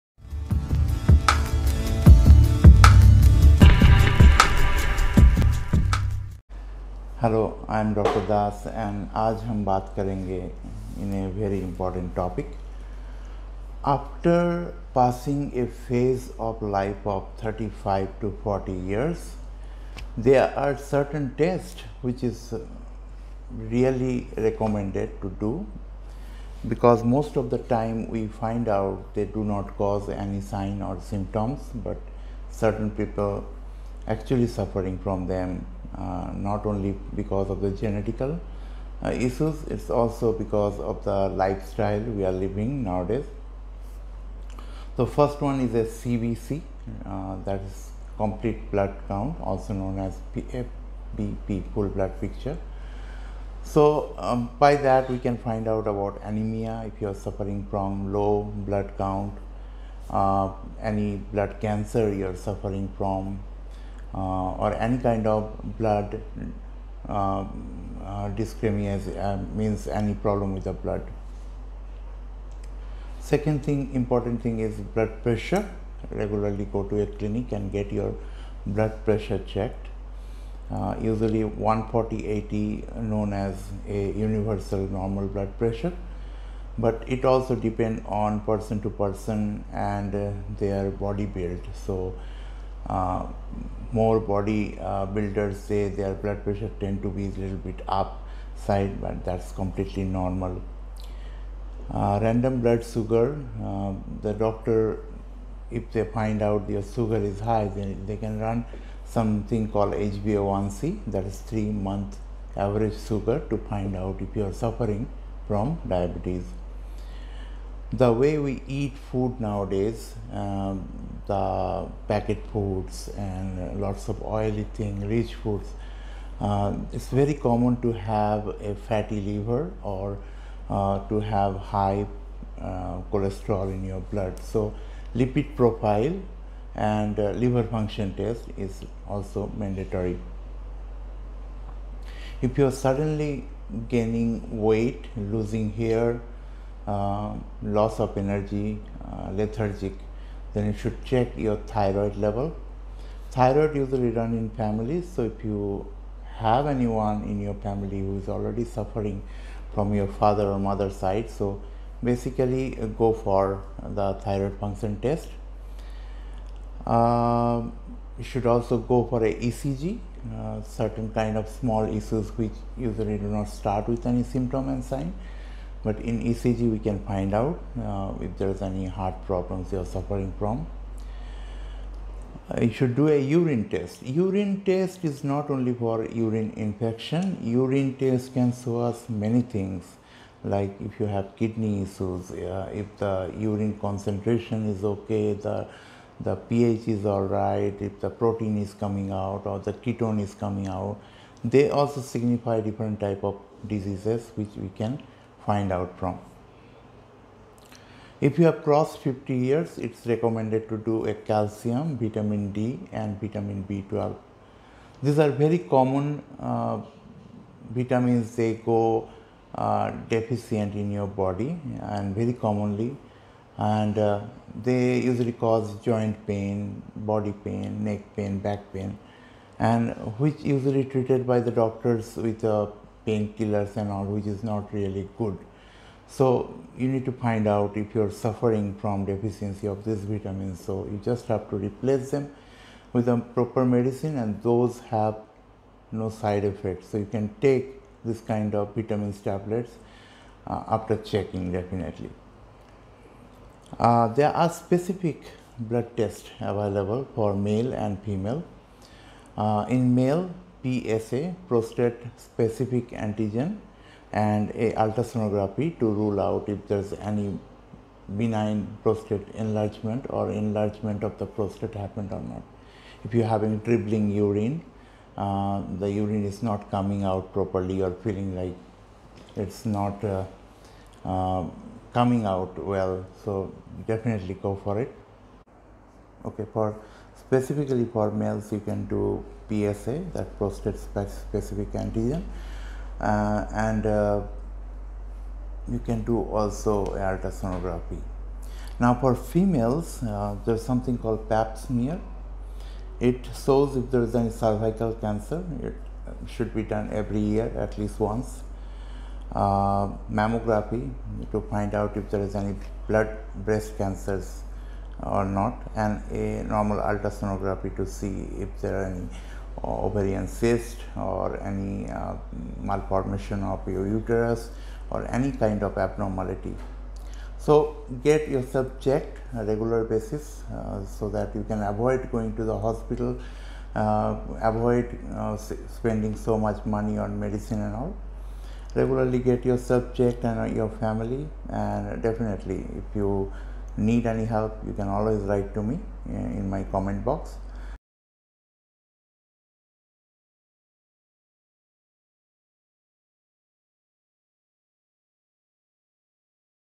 Mandatory tests for adults over 35 years of age
For early detections of certain diseases these tests will help to treat with a better prognosis